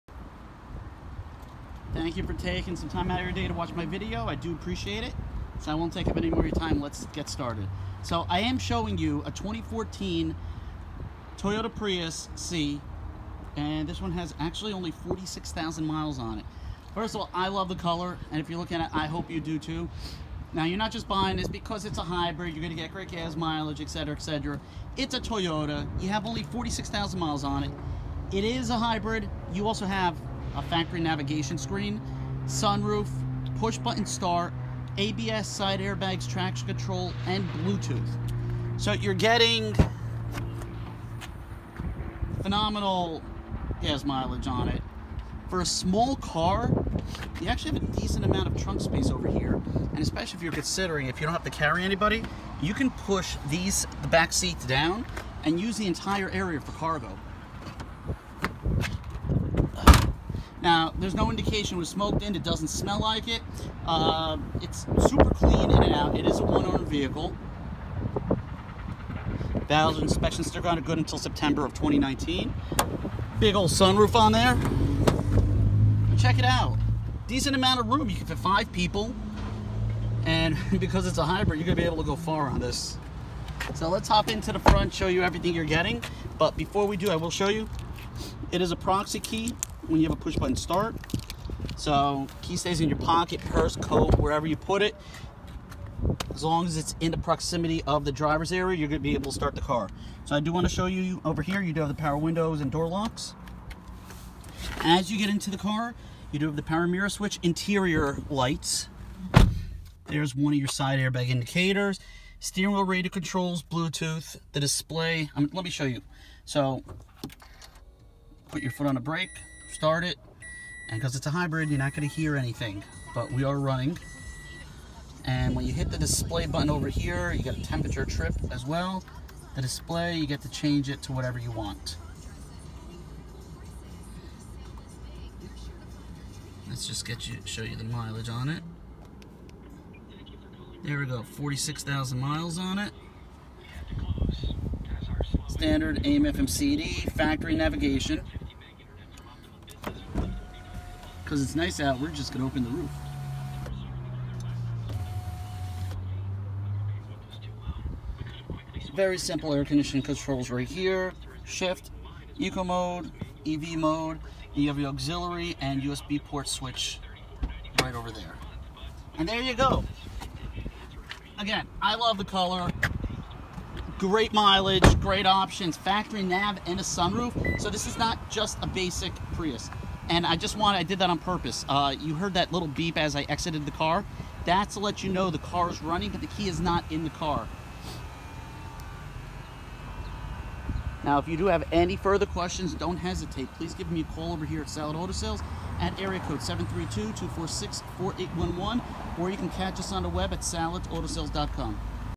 Salit Auto Sales - 2014 Toyota Prius C Level 4 in Edison, NJ
This is a 2014 Prius C with package 4 on it has only 46,000 low miles on it. This low mileage hybrid is certified by Carfax to be a one owner vehicle. It was never smoked in and comes equipped with a Toyota factory power sunroof, navigation, GPS, bluetooth, push button start, ABS, side airbags, traction control and a remote keyless entry. Financing is available.